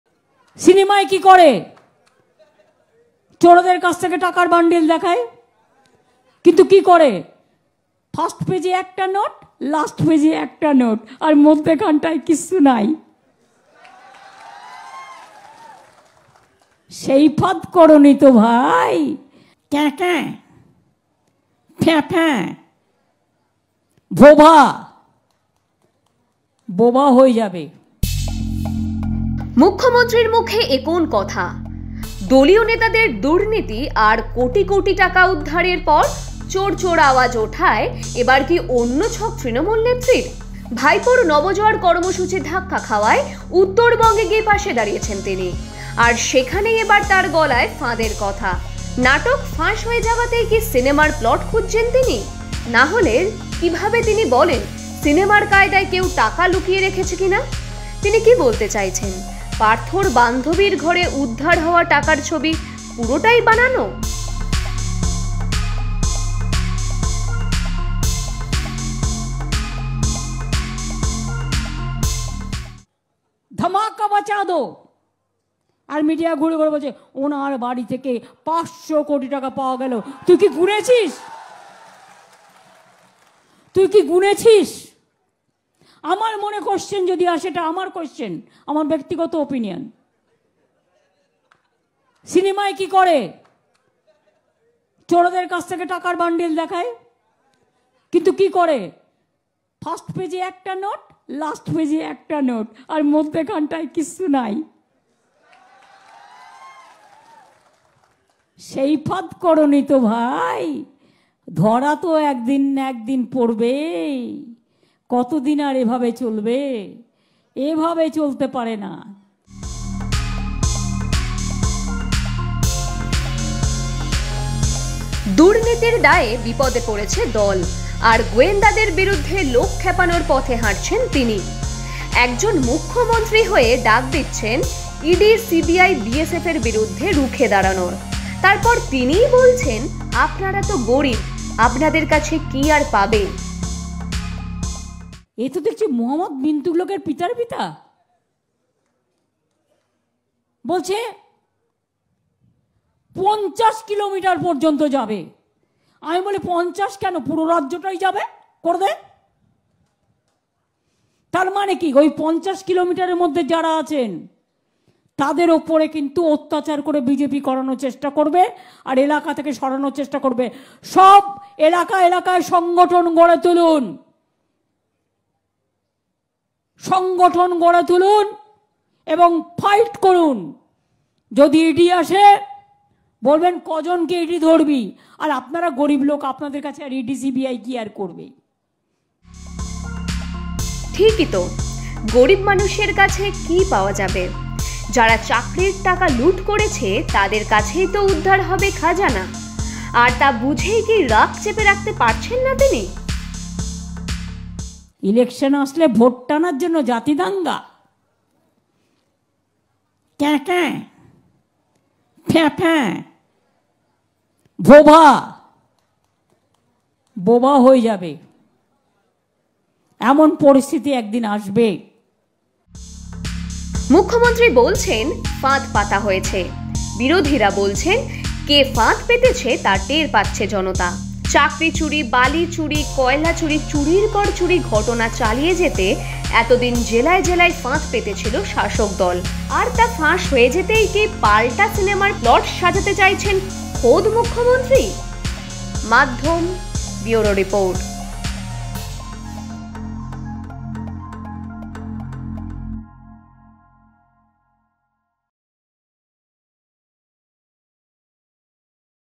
Mamata Banerjee: চুরি (Scam) ধরা পড়তেই মমতার মুখে কেন সিনেমার (Cinema) প্লট?
মুখ্যমন্ত্রীর (Mamata Banerjee) মুখে এ কোন কথা? দলীয় নেতাদের দুর্নীতি (Scam) আর কোটি কোটি টাকা উদ্ধারের (Money Recovery) পর চোর চোর আওয়াজ ওঠায় এবার কি অন্য ছক তৃণমূল নেত্রীর? ভাইপোর নব জোয়ার কর্মসূচি ধাক্কা খাওয়ায় উত্তরবঙ্গে গিয়ে পাশে দাঁড়িয়েছেন তিনি। আর সেখানেই এবার তাঁর গলায় ফাঁদের কথা। নাটক ফাঁস হয়ে যাওয়াতেই কি সিনেমার প্লট (Cineme Plot) খুঁজছেন তিনি? না হলে কীভাবে তিনি বলেন, সিনেমার কায়দায় কেউ টাকা লুকিয়ে রেখেছে কিনা? তিনি কি বলতে চাইছেন, পার্থর বান্ধবীর ঘরে উদ্ধার হওয়া টাকার ছবি পুরোটাই বানানো?

Why CM Mamata Banerjee speaks about trap and cinema plot regarding Bengal Scam money recover case?